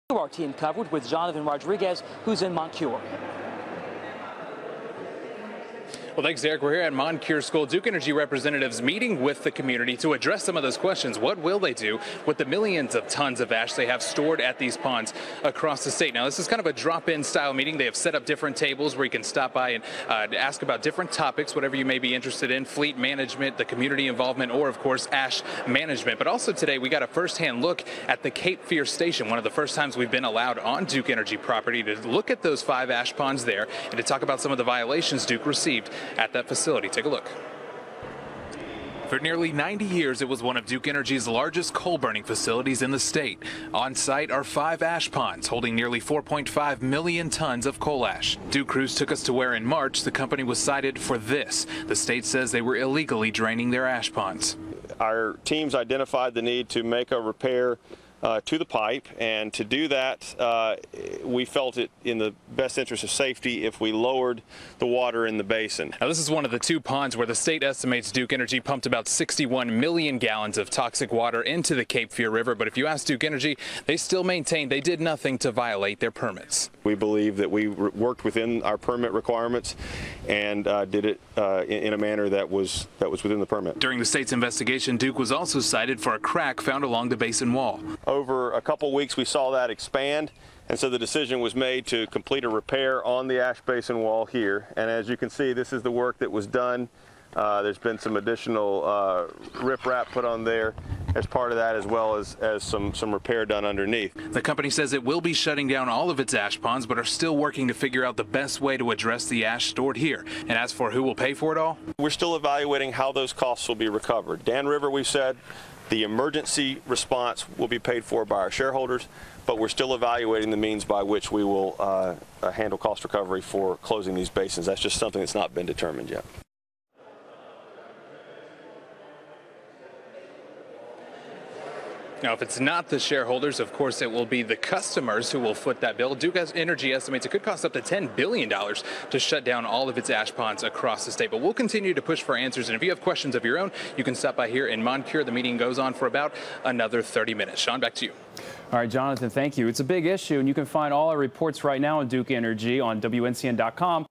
WNCN tours Duke Energy's Cape Fear Plant
On Thursday, May 8, WNCN toured the outside of Duke Energy's Cape Fear Plant in Moncure. The plant is currently in the process of being decommissioned after Duke Energy retired the site in 2012.